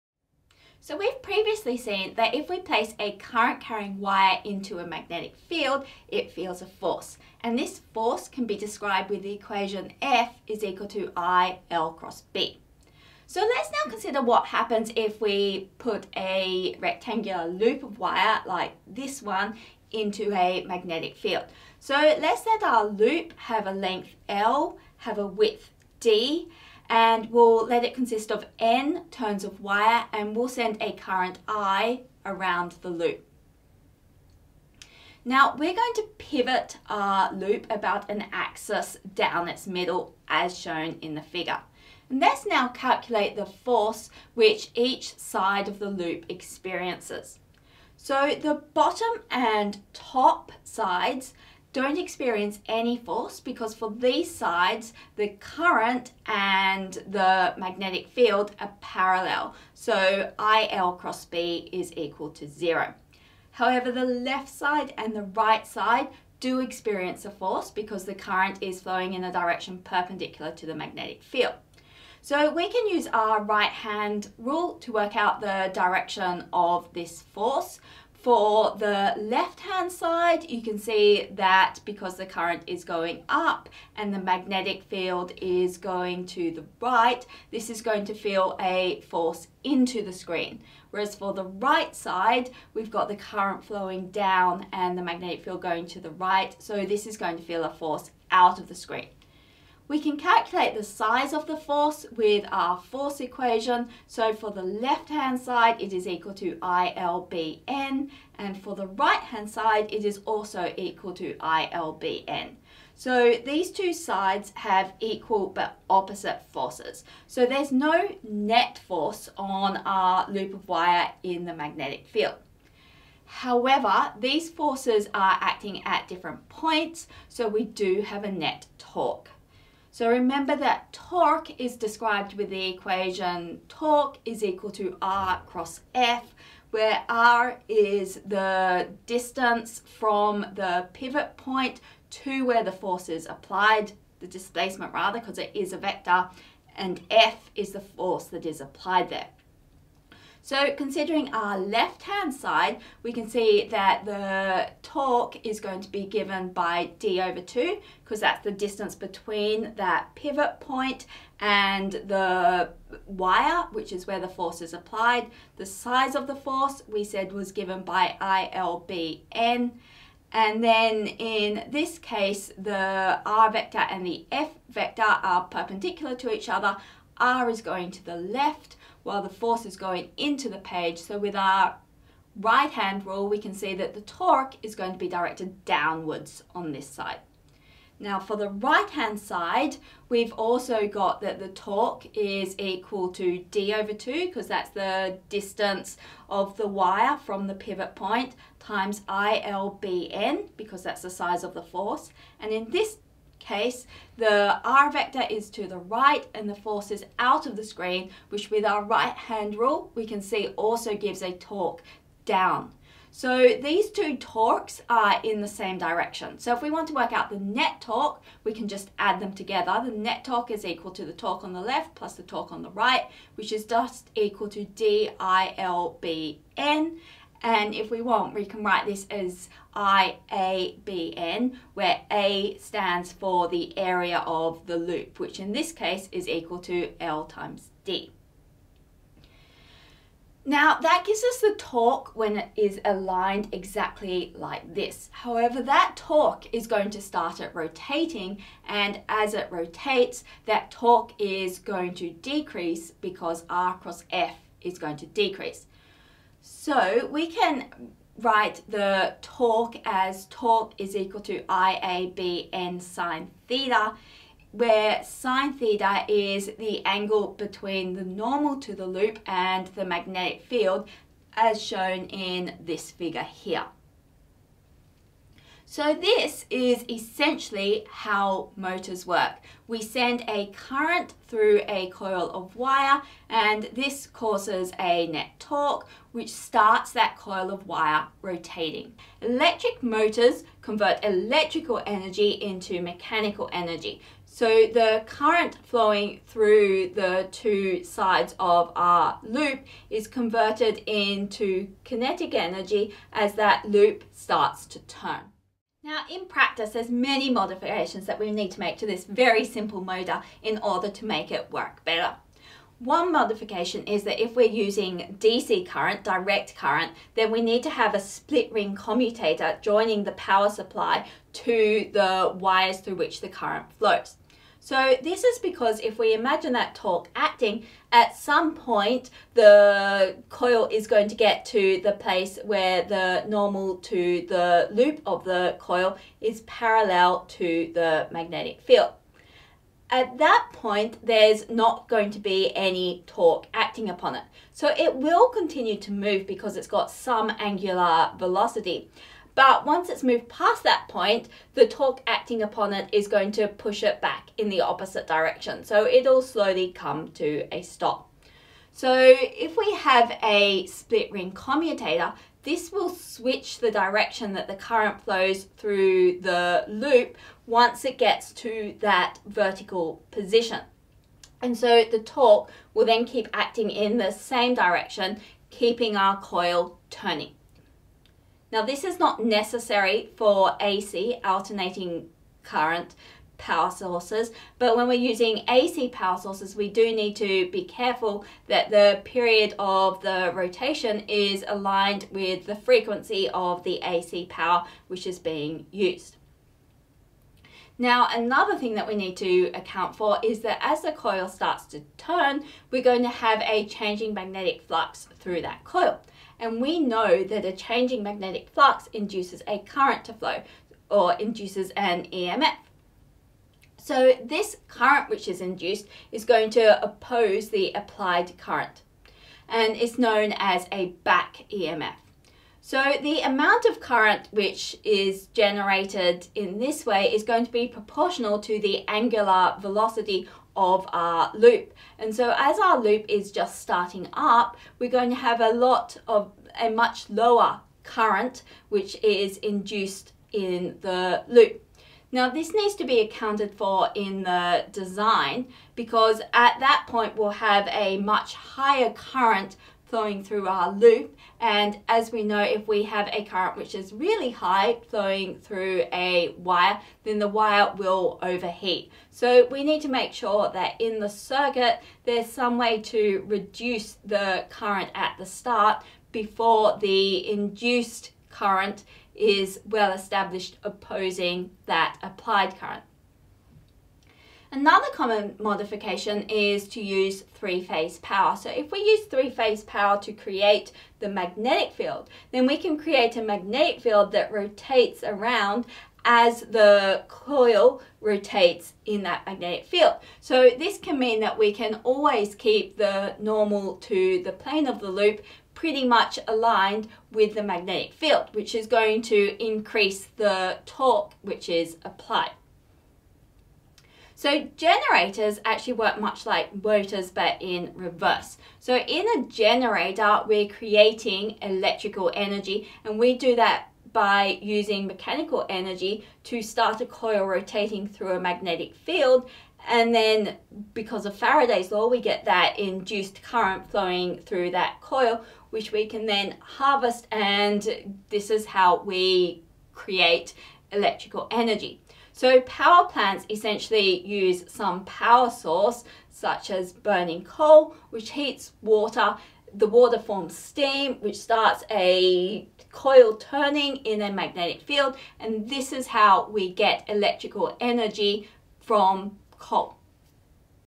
Electric motors
This video looks considers how a current flowing through a coil in a magnetic field causes a torque. This effect is used in electric motors to convert electrical energy into mechanical energy. This fits into lecture 16 for Physics 1B.